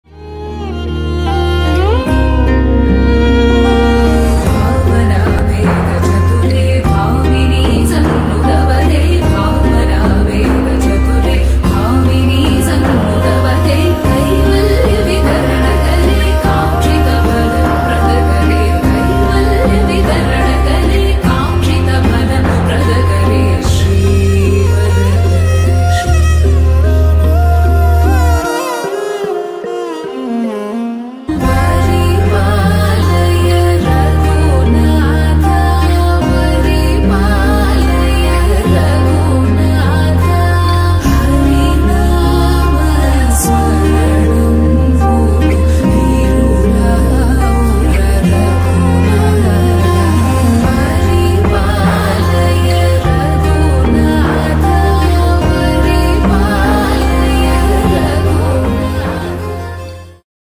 ಶ್ರೀಜ್ಞಾನಶಕ್ತಿ ಸುಬ್ರಹ್ಮಣ್ಯಸ್ವಾಮಿಯ ಮಹಾ ರಥೋತ್ಸವ | MahaRathotsava of Shri Jnanashakthi Subrahmanyaswami
ಶ್ರೀ ಜ್ಞಾನಶಕ್ತಿ ಸುಬ್ರಹ್ಮಣ್ಯಸ್ವಾಮಿಯ ಮಹಾ ರಥೋತ್ಸವ..

Glimpses of rituals held on occasion of Annual Festival of Pavanje Shri Jnanashakthi Subrahmanyaswami.